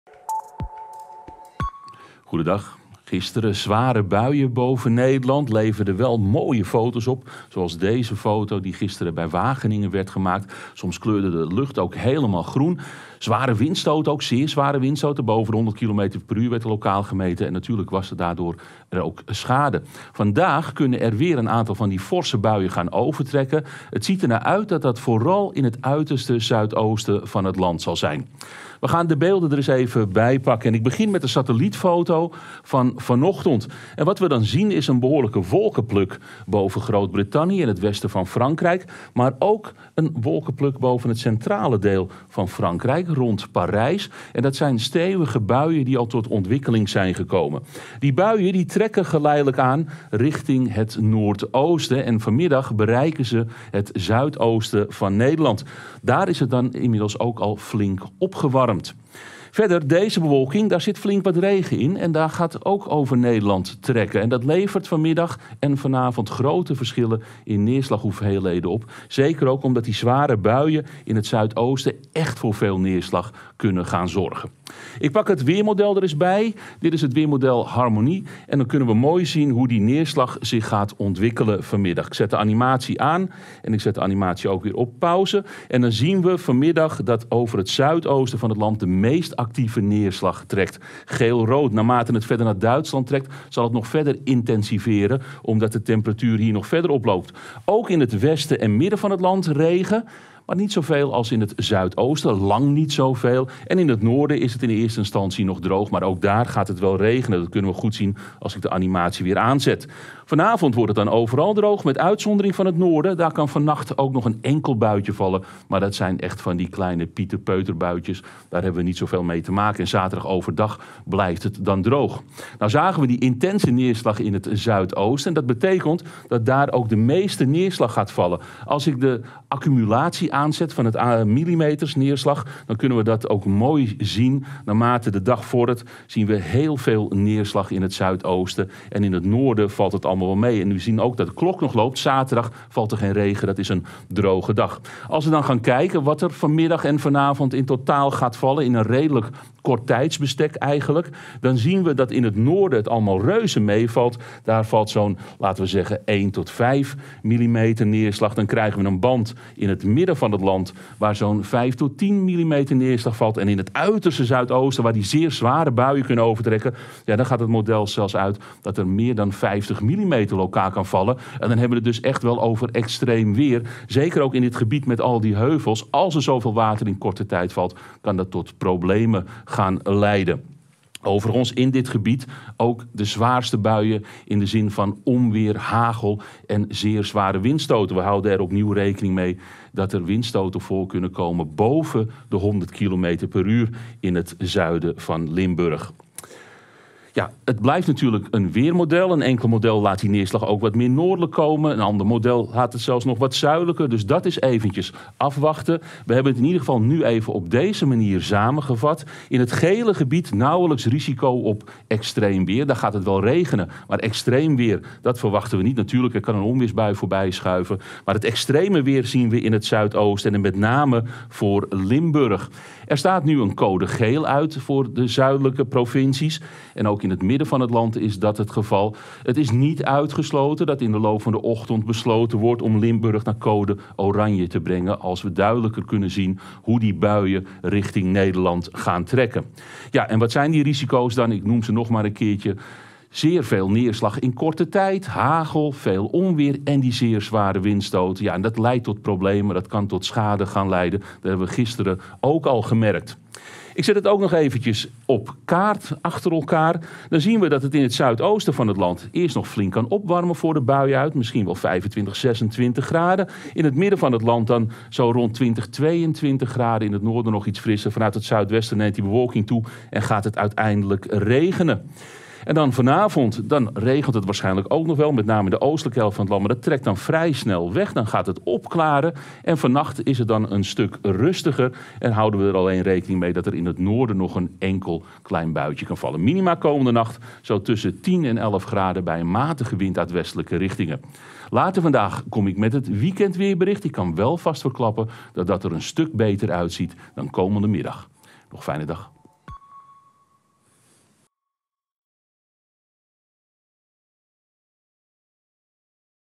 Extra weerbericht: mogelijk extreem weer in het zuidoosten!
Na de heftige onweersbuien van gisteren staat er vandaag opnieuw extreem weer op het programma, vooral voor het zuidoosten van het land. Raymond Klaassen vertelt je alle details!

UPDATE: 20 mei 09:20: Voor Limburg is nu code oranje al afgegeven.